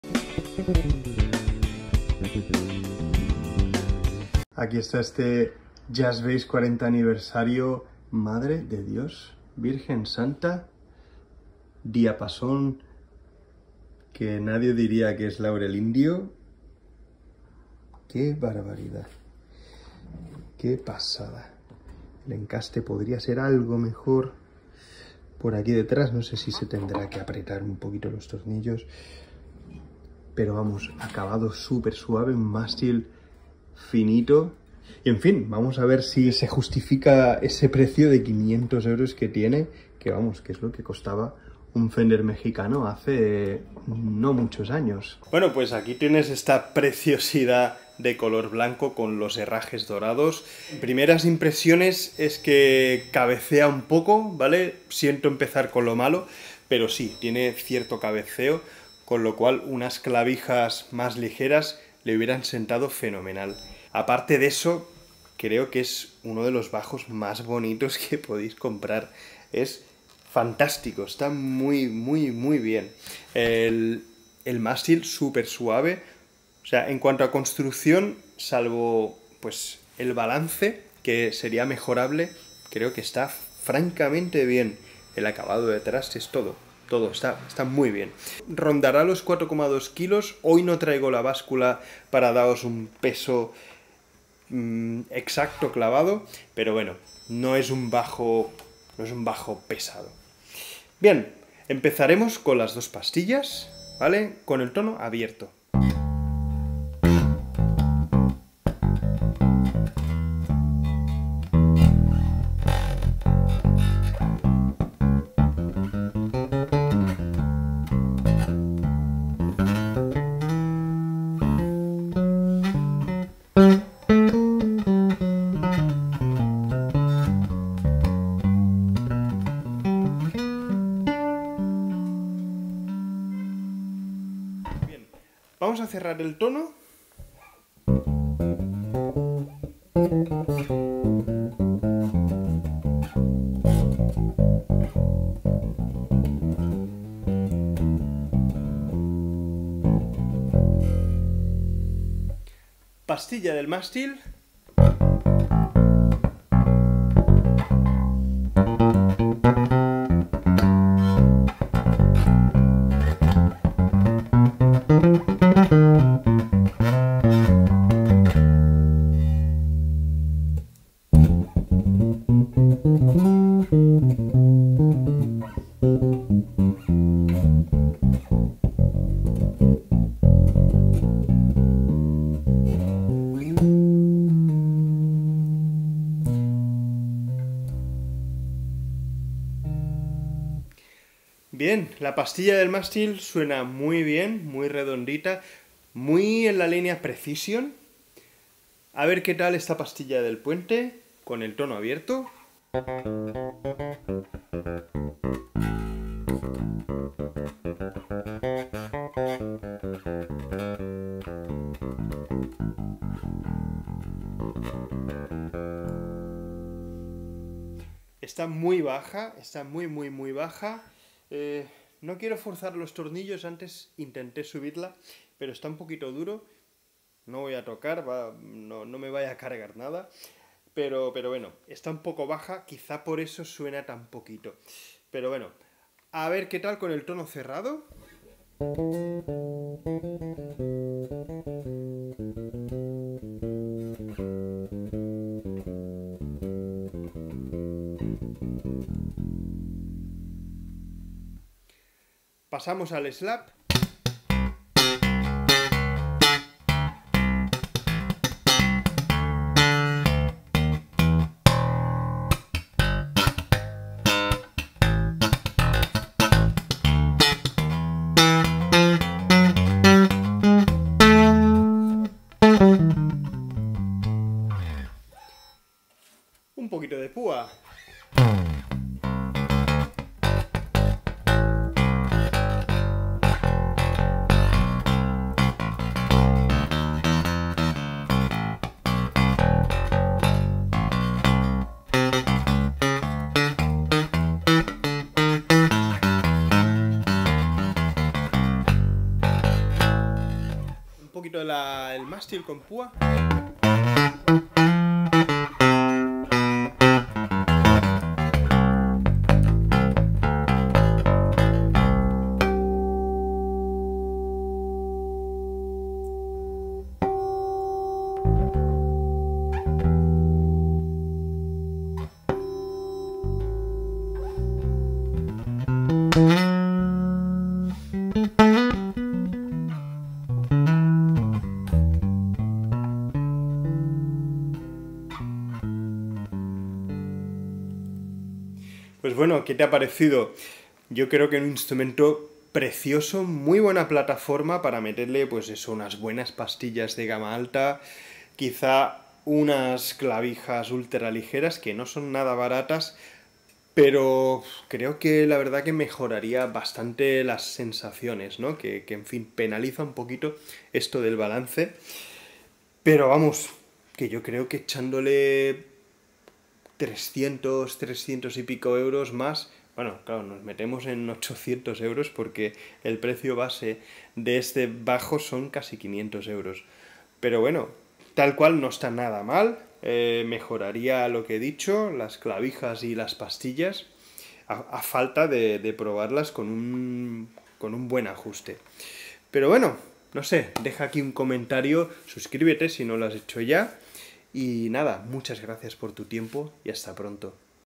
Squier 🍰40th🍰 Anniversary Jazz Bass Gold Edition OW [🇺🇸ENG🇬🇧 subs]
Aquí os dejo este análisis del Squier 40th Jazz Bass OW, una preciosidad a todas luces. Espero que os guste.